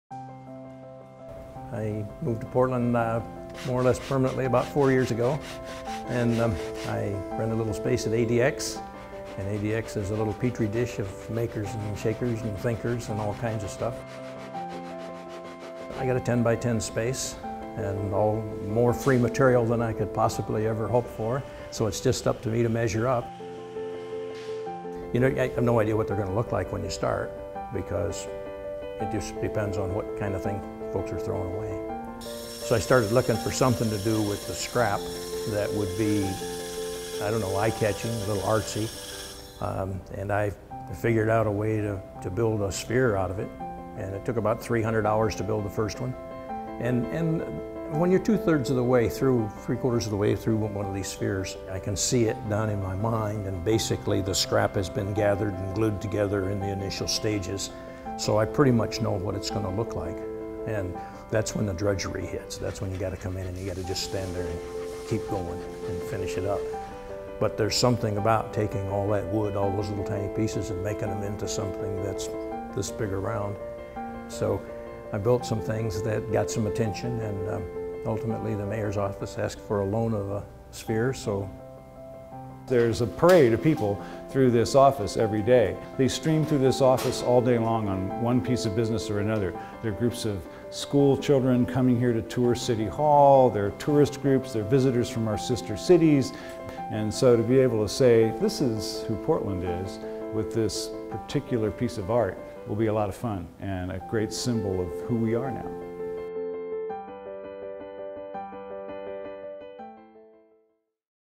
ADX Member Profile - Charlie Haughey
Charlie Haughey is one of ADX Portland's longest and most beloved members.

Charlie is a master cabinet maker, photographer, poet, harmonica player and now artist! Charlie began building these spheres four years ago and just finished his fifth sphere (the large sphere seen mid-build at the beginning of this video).

Charlie also just published a book of never before seen photographs that he took during the Vietnam War entitled A "Weather Walked In" which is available on the ADX website.

Want to make your own wooden art or share a cup of coffee with Charlie?

Happy Making!